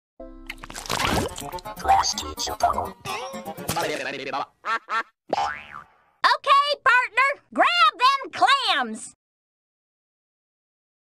Klasky Csupo Robot/Nickelodeon Haypile logos, but Sandy Cheeks voices over the Nickelodeon Haypile
Klasky Csupo Robot/Nickelodeon Haypile logos, but Sandy Cheeks is the voice of the Nickelodeon Haypile.